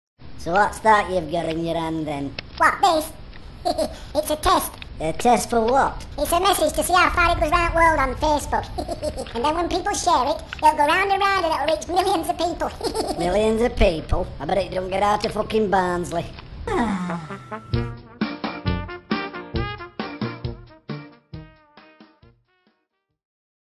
PIT MOUSE STRIKES AGAIN - Titch tells Frankie about his Facebook Test
Just another daft video snippet from PIT MOUSE STRIKES AGAIN